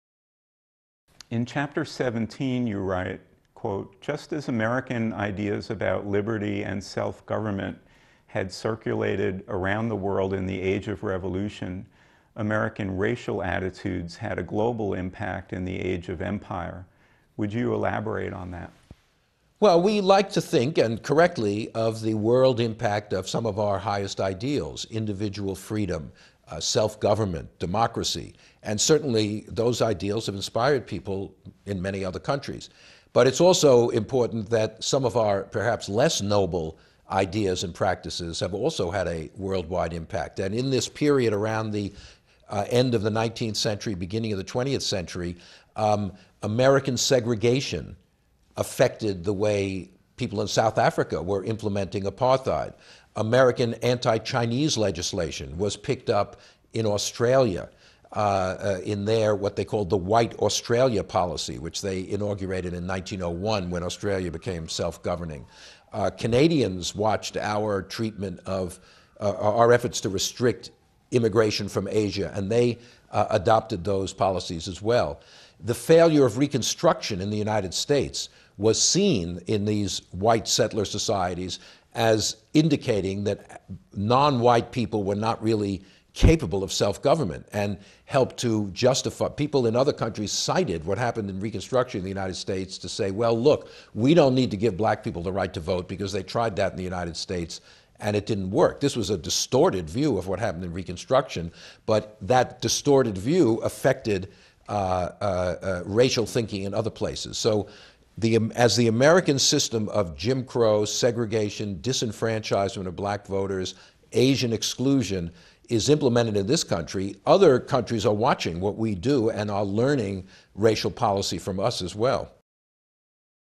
Eric Foner on racism in the age of empire
Interviews with the author of GIVE ME LIBERTY! AN AMERICAN HISTORY

Question:  In Chapter 17 you write, "Just as American ideas about liberty and self-government had circulated around the world in the Age of Revolution, American racial attitudes had a global impact in the age of empire." Would you elaborate on that?